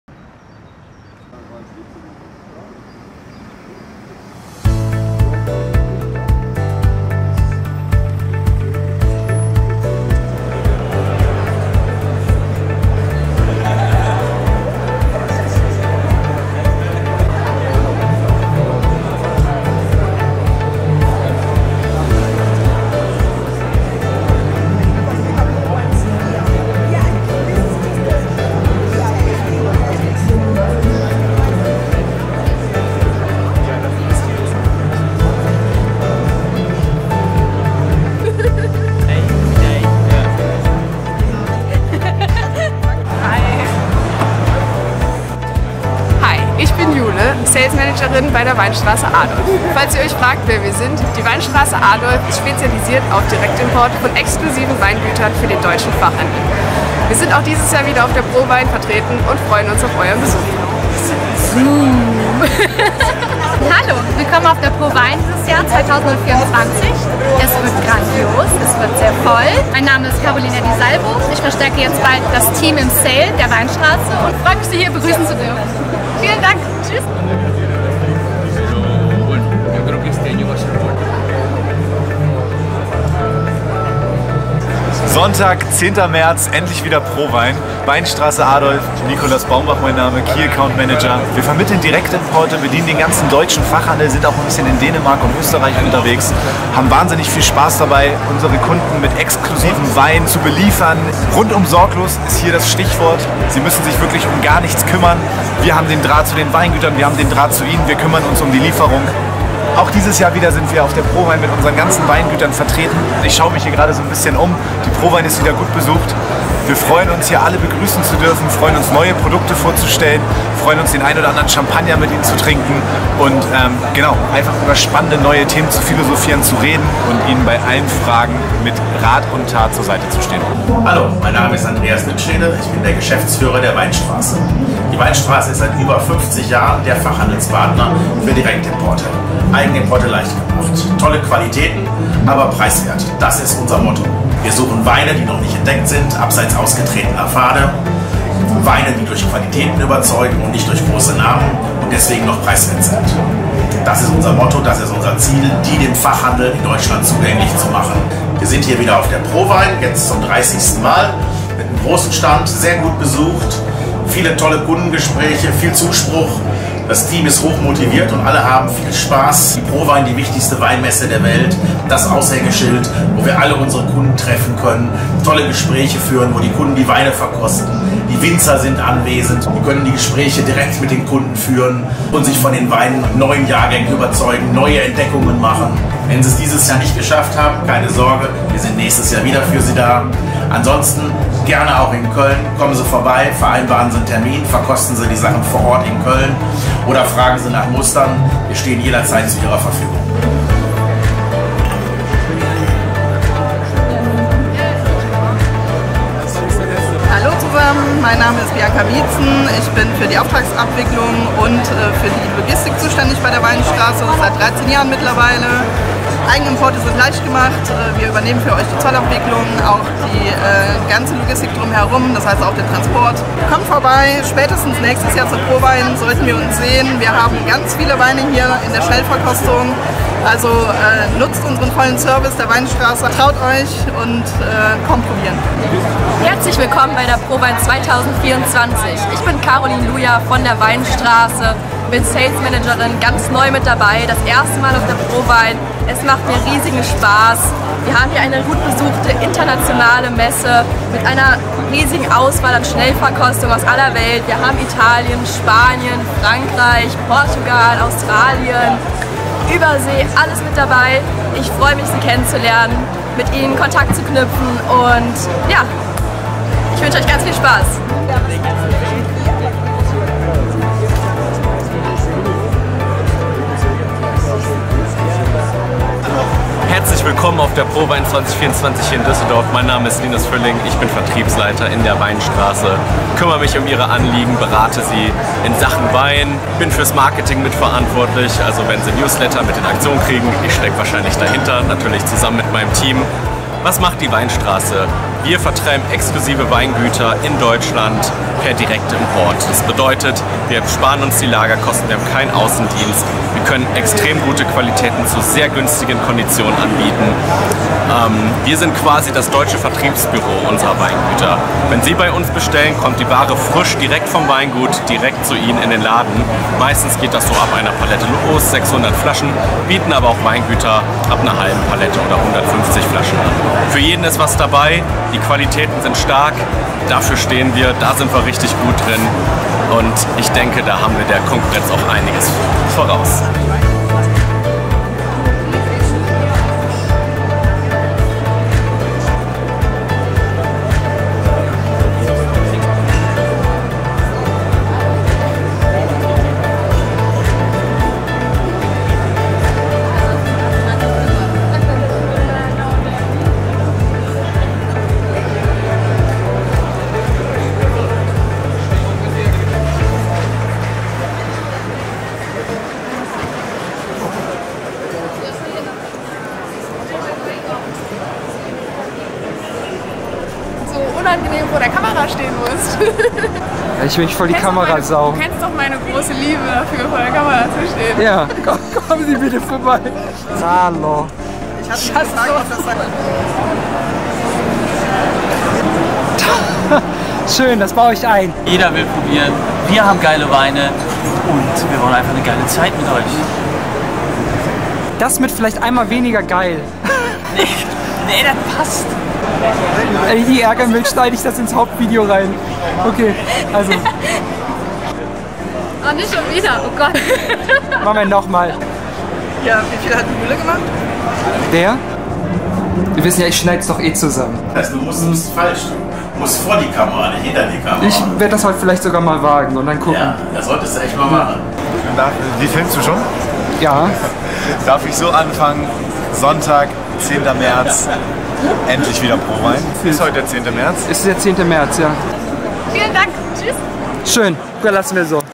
Die Weinstrasse Adolph auf der ProWein 24
Vielen Dank für Ihren Besuch!

Danke für Ihren Besuch an unserem Stand auf der diesjährigen ProWein. Was war dies nur für eine tolle Messe dieses Jahr?! Es war voll, belebt, dynamisch und wir hatten unglaublich viele gute Gespräche. Der Zuspruch und das Interesse an uns und unseren Produkten war unglaublich. Die Messe war erneut ein voller Erfolg und zwar auf ganzer Linie.

Wir haben die Messe wirklich sehr genossen und hatten wahrlich alle Hand voll zu tun. Wir möchten uns an dieser Stelle bei Ihnen bedanken, für die schöne Zeit und dass Sie alle unserer Einladung gefolgt sind und uns an unserem Stand besucht haben. Danke für all die herrlichen, interessanten und guten Gespräche und vor allem für den wunderbaren positiven Zuspruch, den wir auf der Messe für unsere Produkte erfahren durften.

Es ist ein wirklich hochmotivierendes Gefühl, wieder mit so vielen tollen Kunden zusammen sein zu dürfen und sich auszutauschen.

Die Stimmung auf der ProWein war exzellent, so konnten wir uns auf unserem Stand nicht über Langeweile beklagen und hatten wirklich viel zu tun. Wir mussten oftmals an mehreren Tischen gleichzeitig beraten. Wir bitten daher um Entschuldigung, wenn dadurch ggf. einige Wartezeiten entstanden sein sollten. Insgesamt können wir ein sehr positives Fazit ziehen.

Alle, die es dieses Jahr nicht zur Messe geschafft haben, können wir getrost beruhigen: Nach der ProWein ist vor der ProWein! Nutzen Sie Ihre Chance im kommenden Jahr, am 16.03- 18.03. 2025!

Das gesamte Team der Weinstrasse dankt Ihnen für die tollen Tage.
Andreas Dünschede & das Weinstrasse-Team